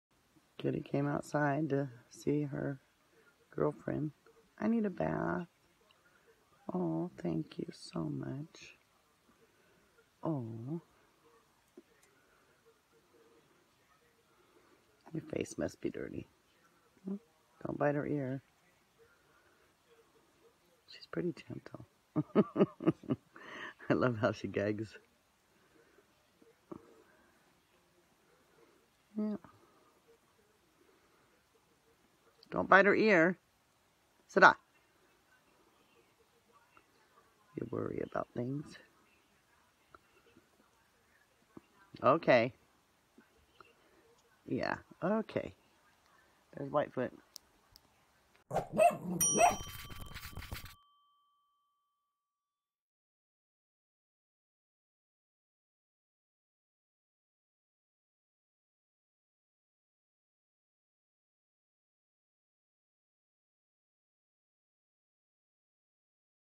Deer gives patient kitty a loving tongue bath
This incredible friendship between a deer and a cat is definitely not something you'll see everyday. Adorable!